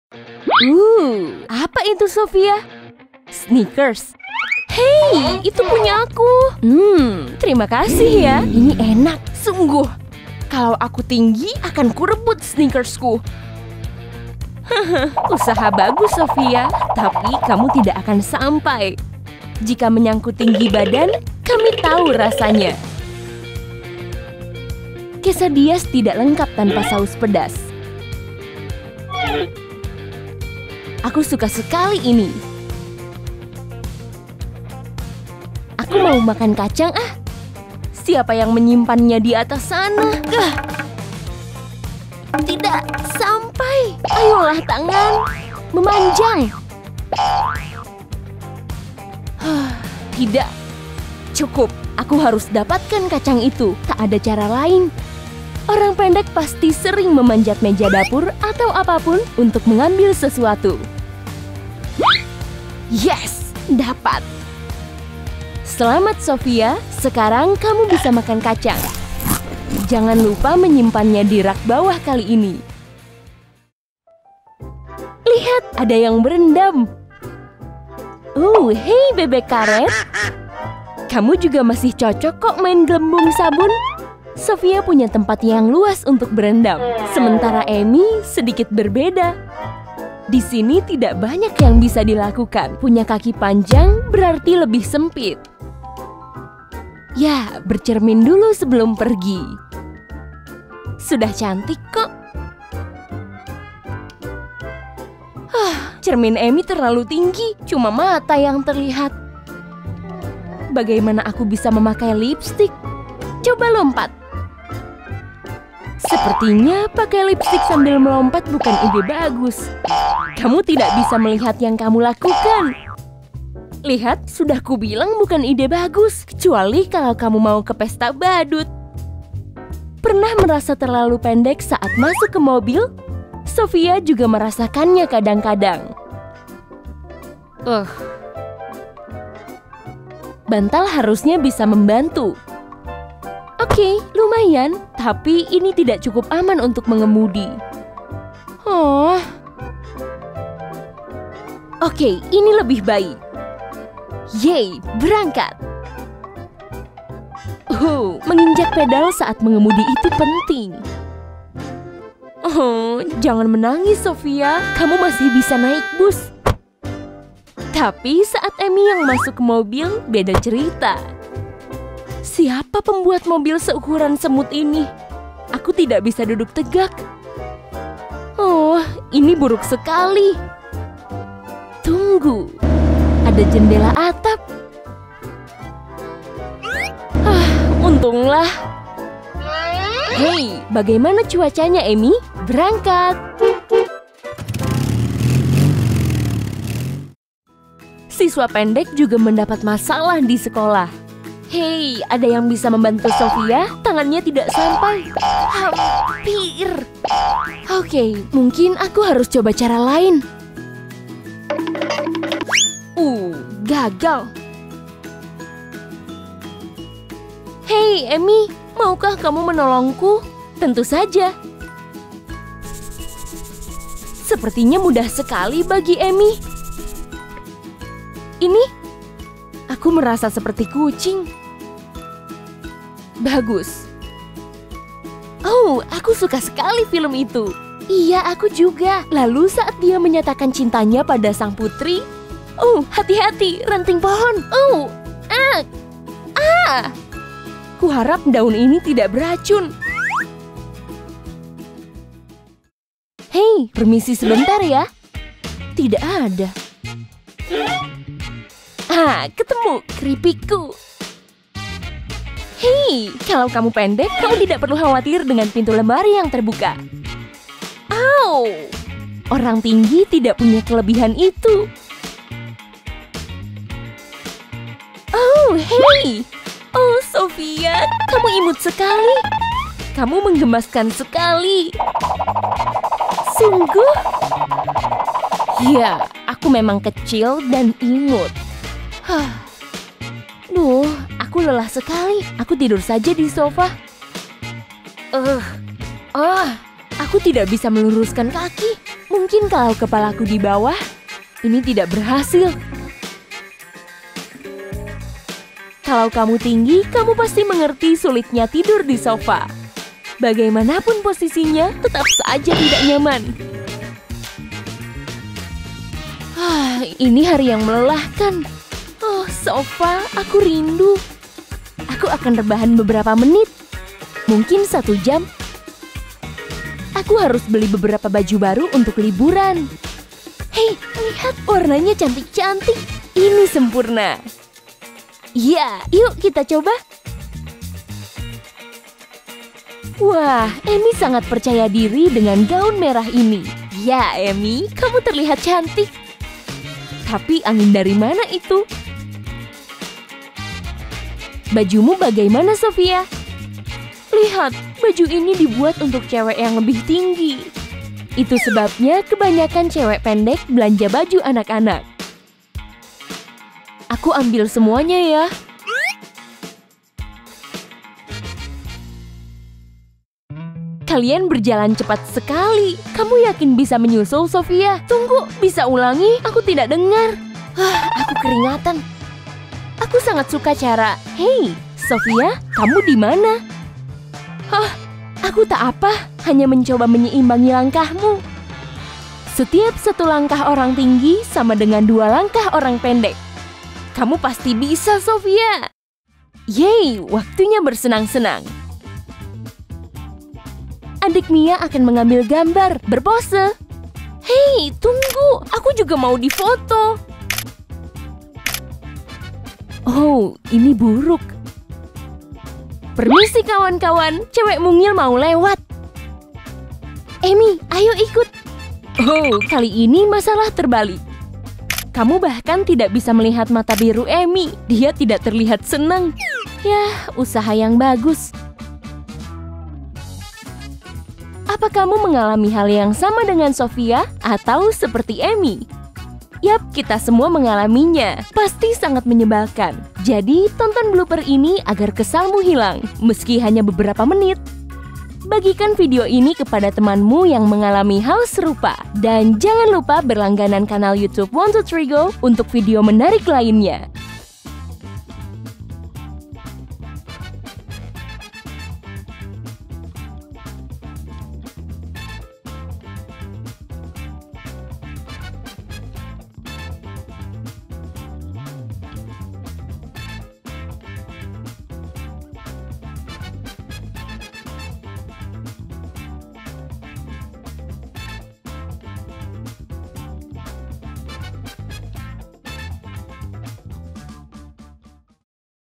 MASALAH ORANG PENDEK VS ORANG TINGGI || Fakta yang Sering Terjadi oleh 123 GO!
Akui saja, terkadang tinggi itu menguntungkan. Tetapi orang-orang pendek juga punya kelebihan sendiri.

Saat berhadapan dengan masalah tinggi badan, kamu tahu rasanya.

Apa kamu mengalami hal yang sama dengan Sophia? Atau seperti Amy?

Bagikan video ini kepada temanmu yang mengalami hal serupa.